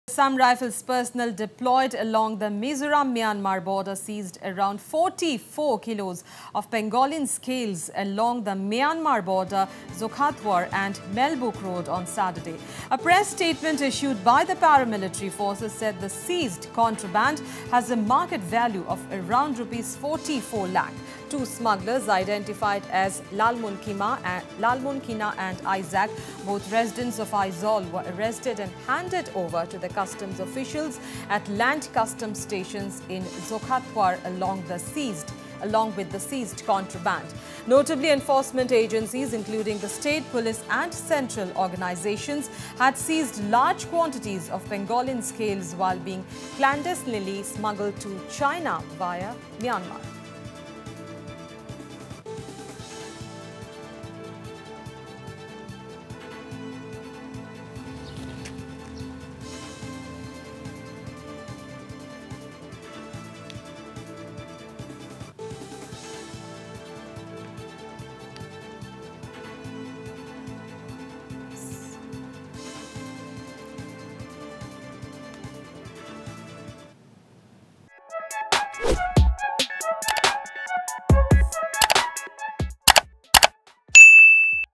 44kg of pangolin scales seized at Mizoram-Myanmar border, 2 held
Assam Rifles personnel seized around 44 kg of pangolin scales along the Myanmar border Zokhawthar and Melbuk road on Saturday. A press statement issued by the paramilitary forces said the seized contraband has a market value of around Rs 44 lakh.

Two smugglers identified as Lalmuankima and Isaac, both residents of Aizawl were arrested and handed over to the Customs officials at Land Customs Station in Zokhawthar along with the seized contraband.